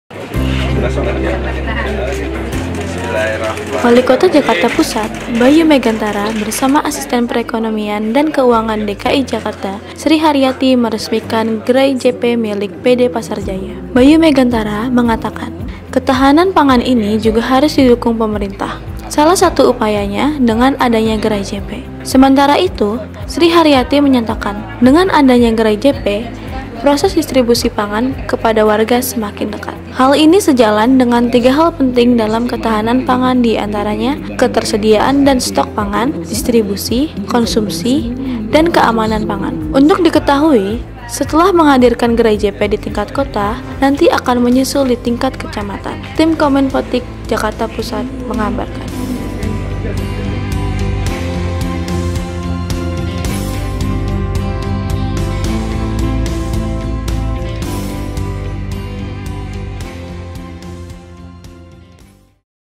Peresmian “GeraiJP” Jakarta Pusat.
Wali Kota Jakarta Pusat, Bayu Meghantara bersama Asisten Perekonomian dan Keuangan DKI Jakarta, Sri Haryati meresmikan Gerai JP milik PD Pasar Jaya. (5/8)